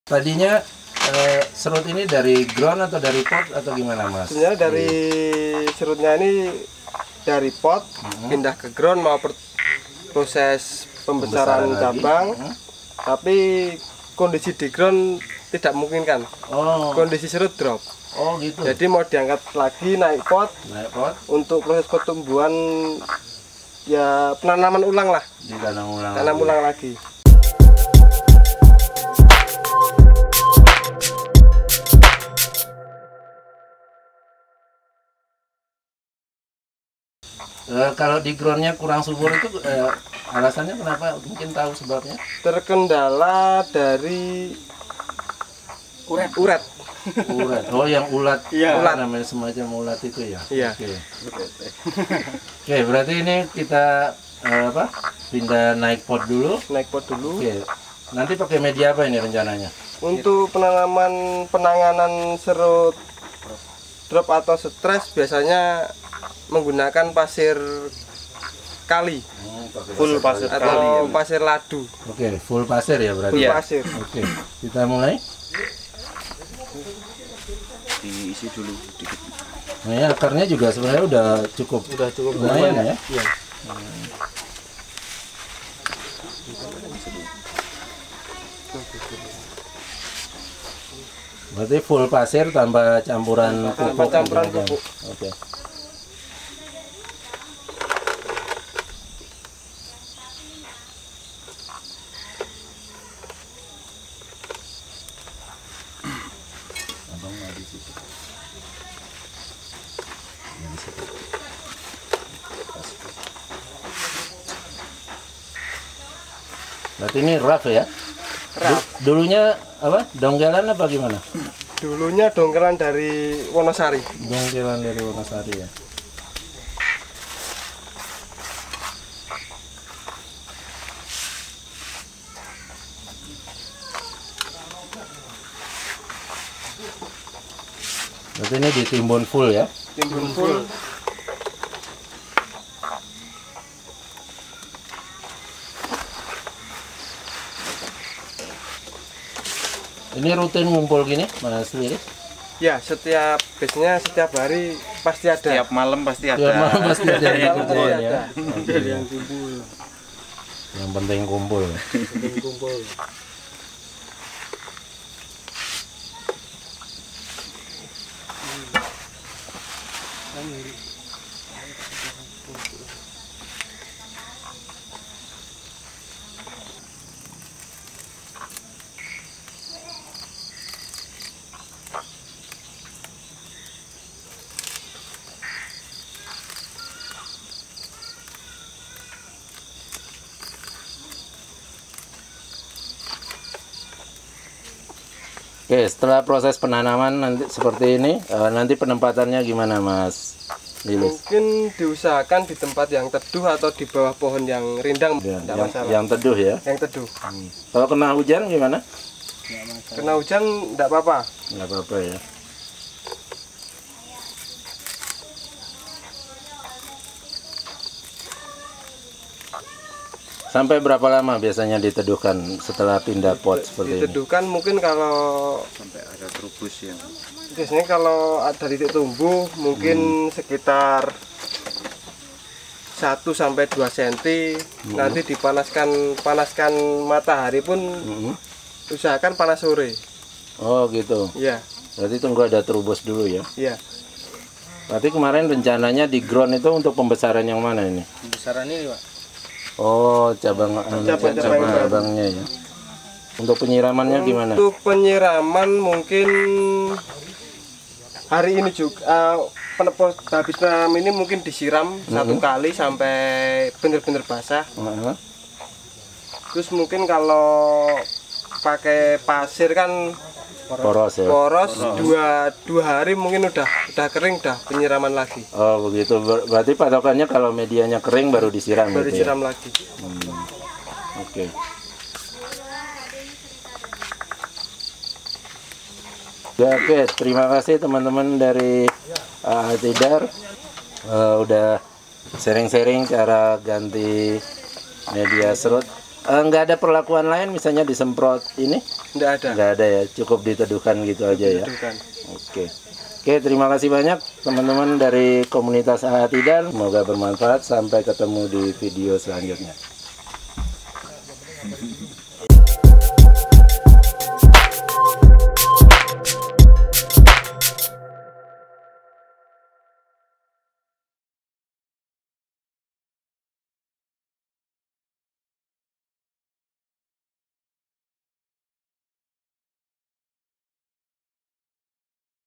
Bahan Bonsai Serut Dari Ground Naik Pot Bersama Komunitas AATIDAR Magelang
Bahan Bonsai Serut Naik Pot Dari Ground Bersama Komunitas AATIDAR Magelang